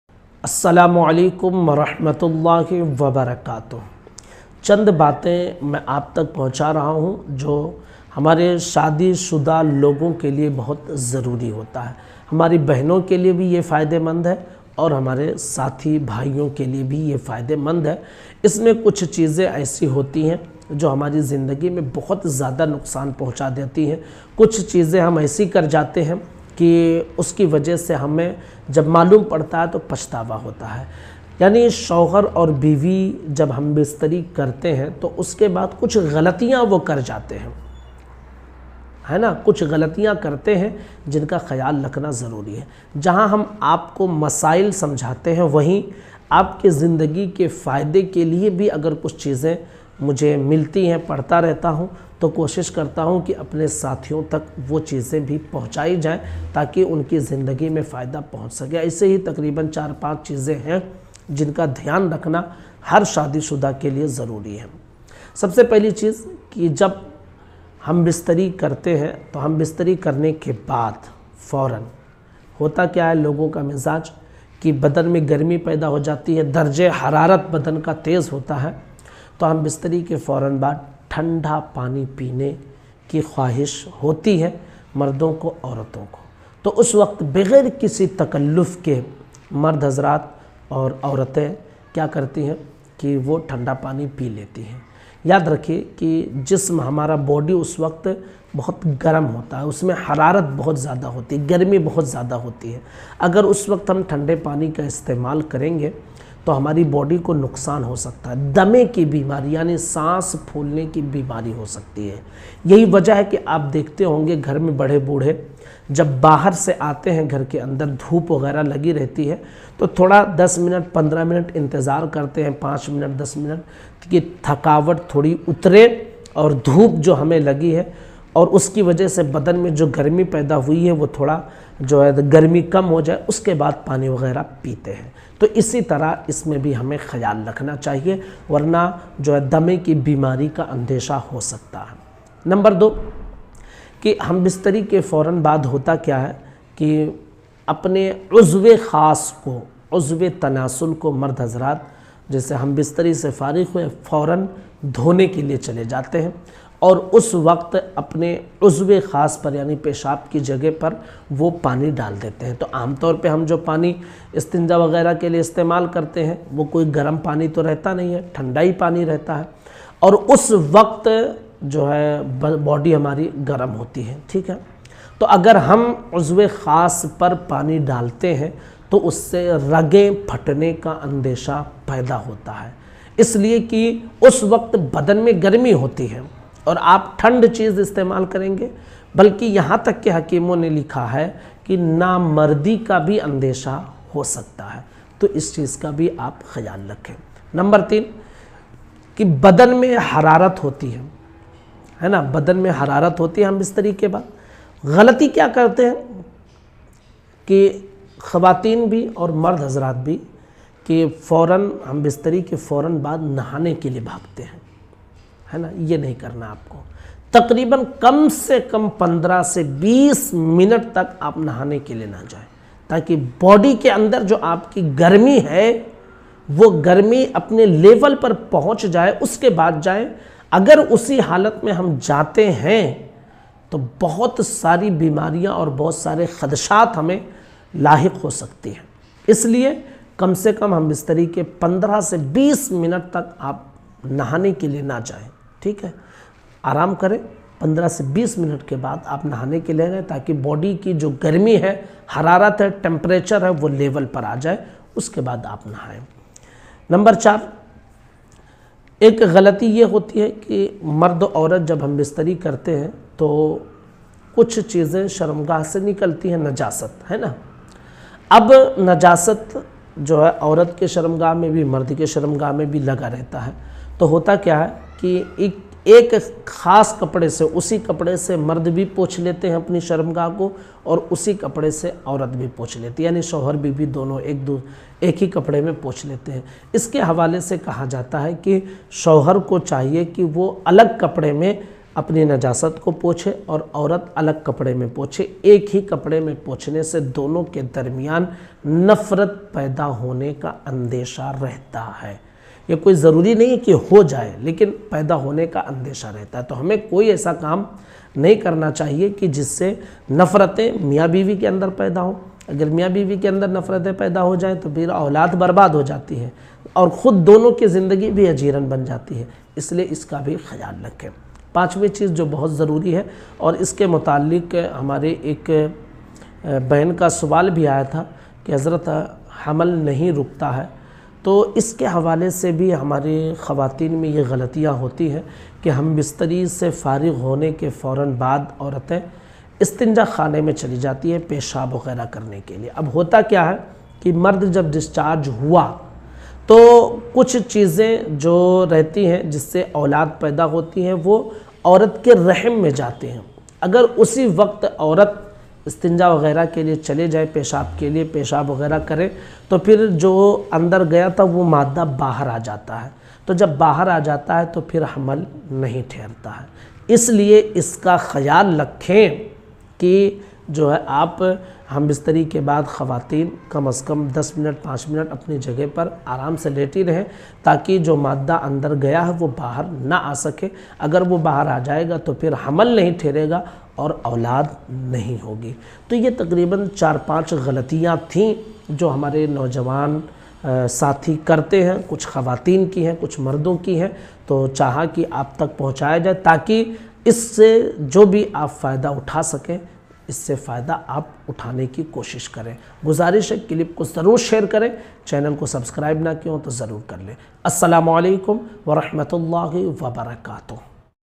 ये 5 गलती जिन्दगी में कभी भी ना करना वरना पछताना होगा || Ye 5 Galati Kabhi Na Karna Warna pachchtawa
Zindagi me kabhi bhi ye 5 Kaam Na Karna Warna pachchtawa hoga